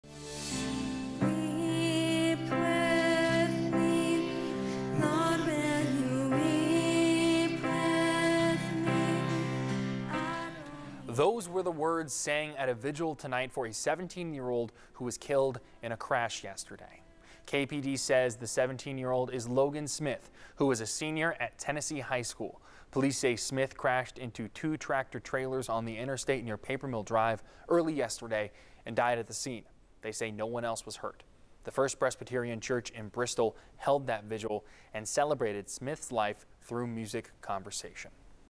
Vigil held for teen killed in I-40W crash on Saturday
KPD says the 17-year-old is Logan Smith who was a senior at Tennessee High School.